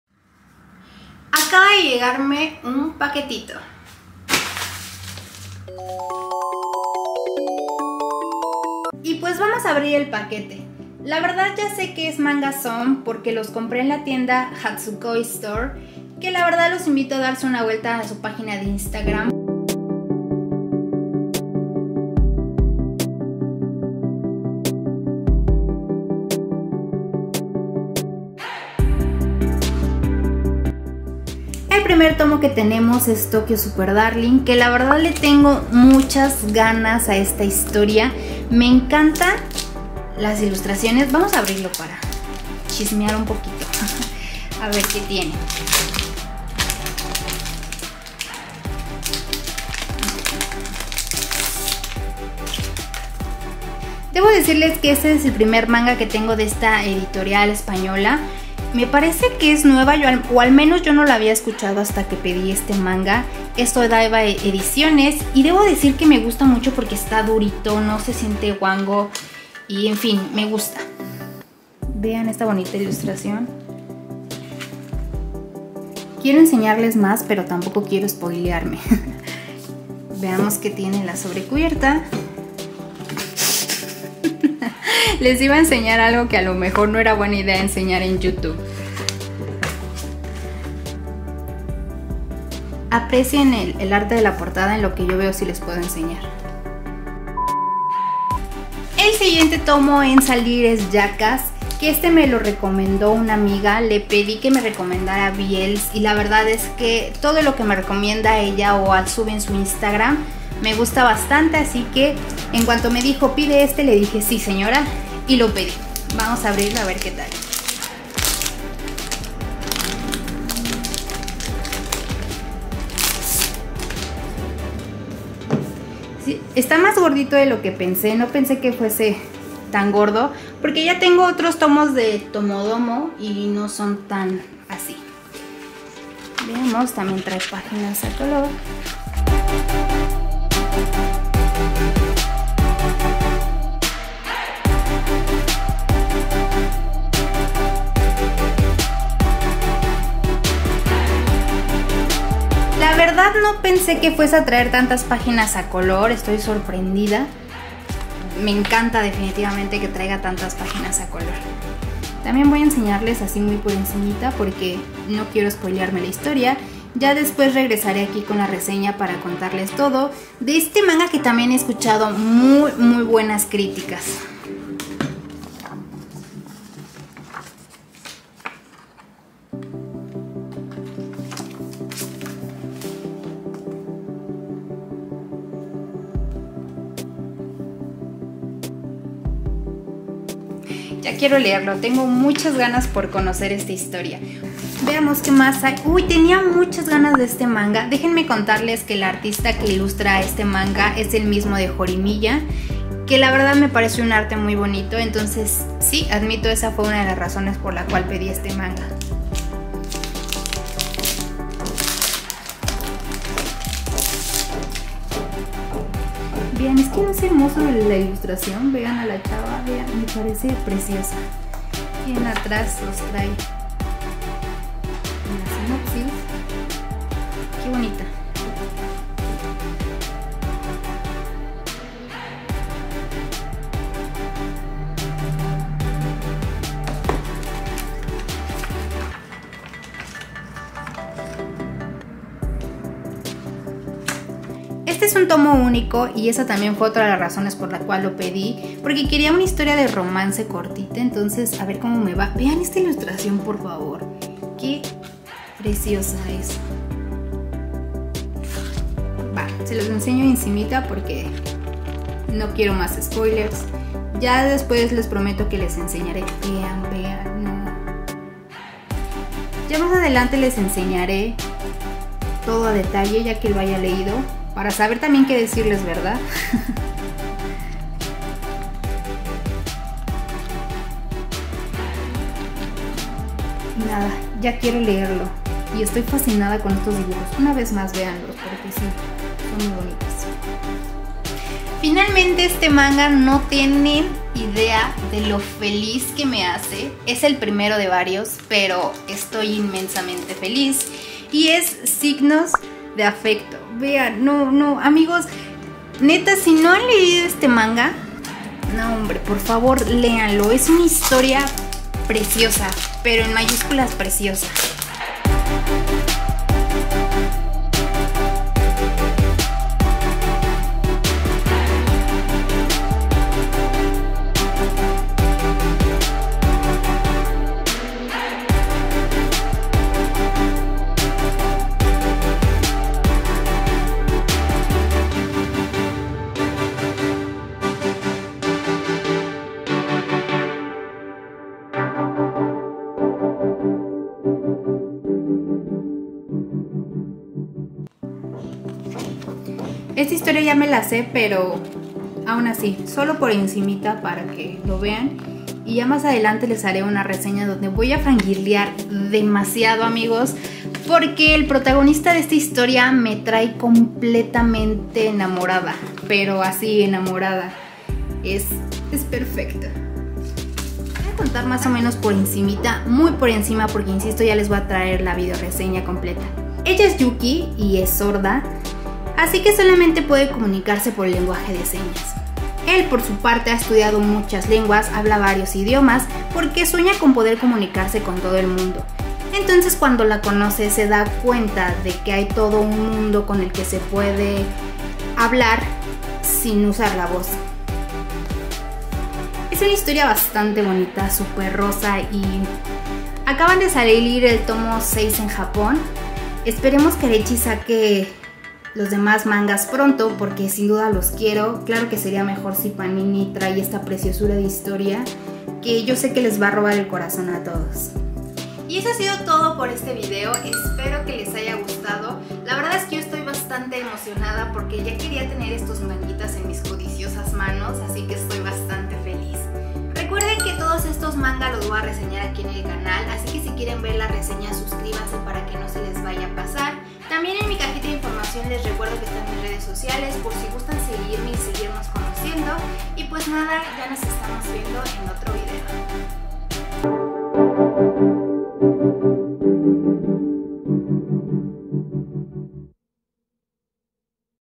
Unboxing desde España 💕
¡Hola, otaku banda!
(灬º‿º灬)♡

El día de hoy les traigo un mini unboxing de unos mangas que me llegaron de España a los cuales les tenía muchísimas ganas 💕😍

💕 ¡VAMOS A SEGUIRNOS! 💕